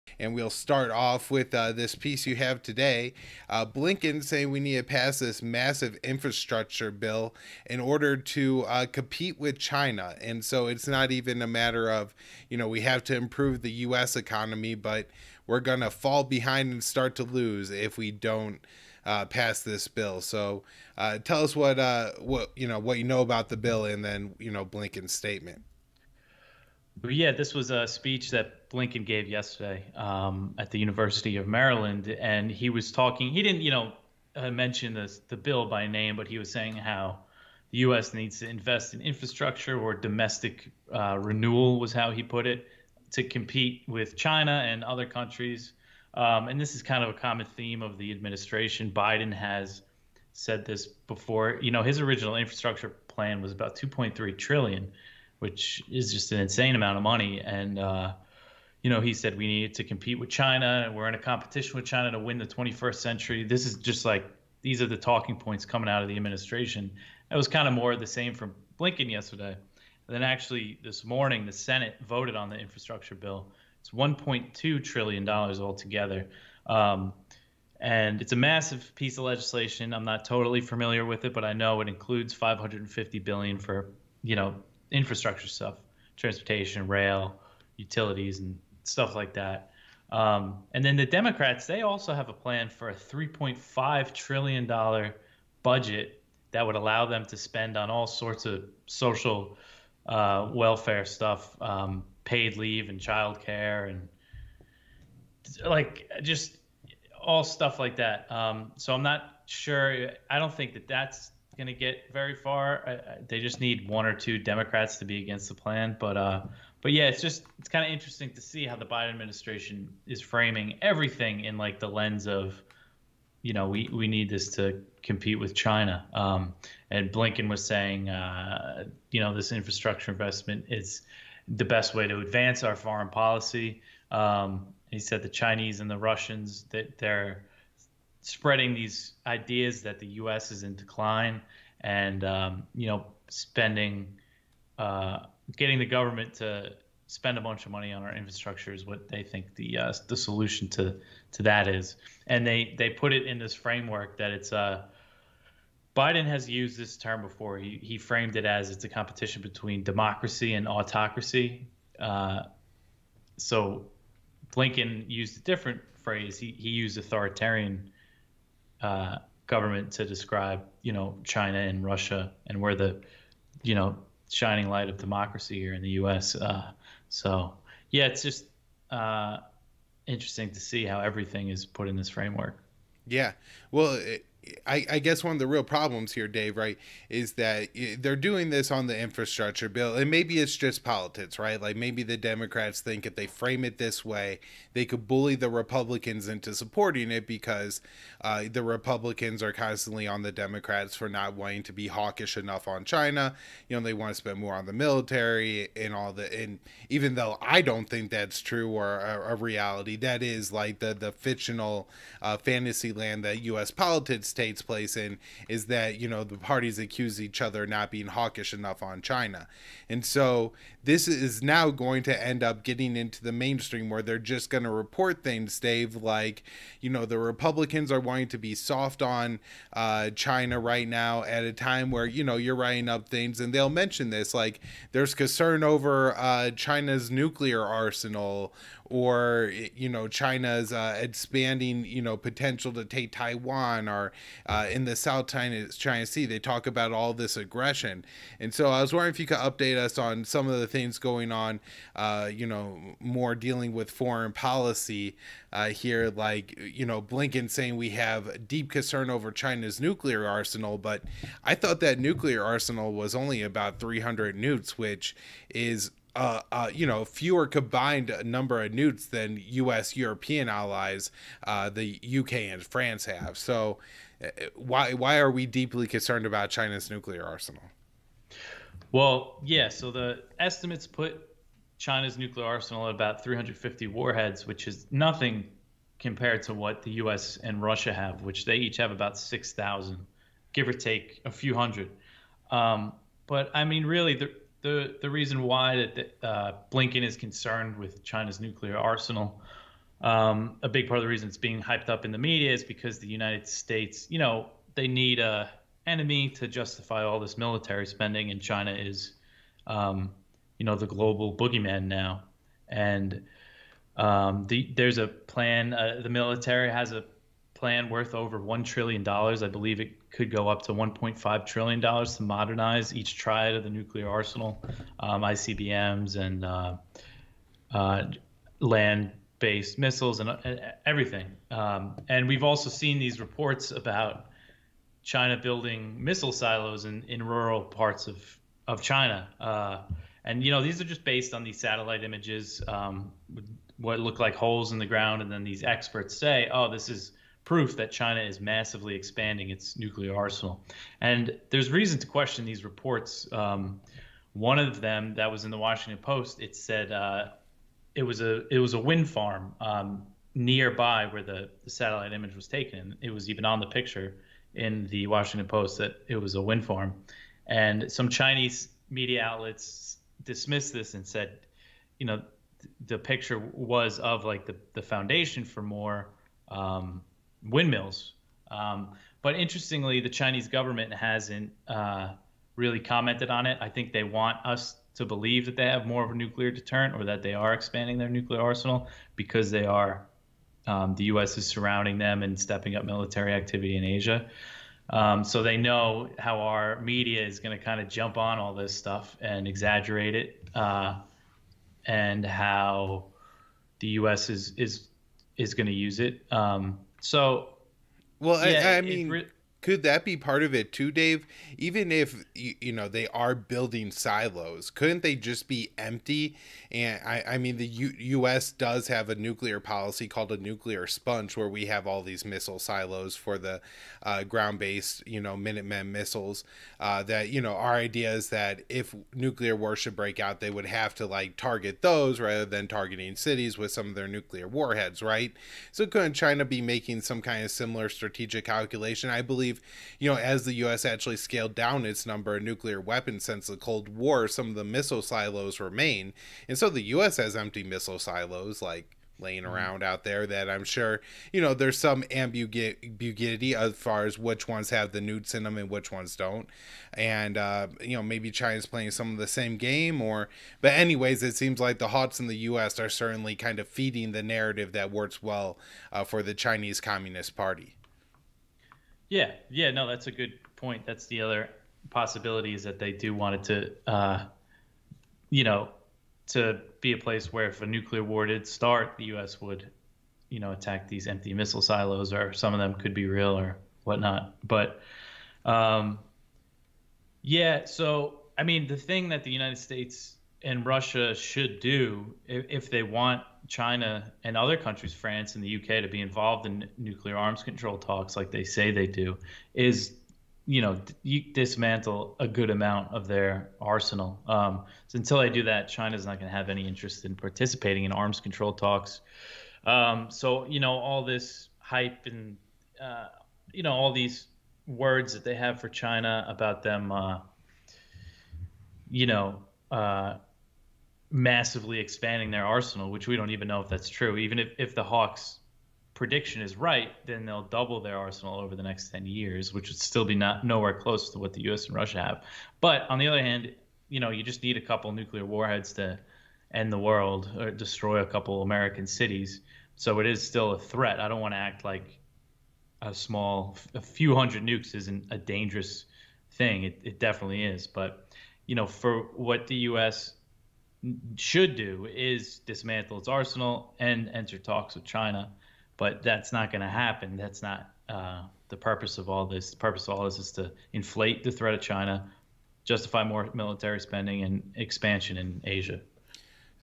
COI #147 CLIP: Biden Officials Leverage ‘China Threat’ to Push Republicans on Infrastructure Bill
In this clip from COI #147, Dave DeCamp – News Editor at Antiwar.com – returns to the show to discuss how American pols increasingly use China as a political weapon. Secretary of State Antony Blinken recently warned that the US must pass a massive infrastructure spending bill or risk ‘losing’ to China. He also warned that Beijing is in the midst of a nuclear buildup. However, the Chinese nuclear arsenal is many times smaller than the American arsenal, and the US is planning to spend trillions updating its own nuclear weapons stockpile.

Odysee

Rumble

bTTEiLoteVdMbLS7YqDVSZyjEY1eMgW7CP

36PP4kT28jjUZcL44dXDonFwrVVDHntsrk

qp6gznu4xm97cj7j9vqepqxcfuctq2exvvqu7aamz6

MeWe

Apple Podcast

Google Podcasts

Amazon Music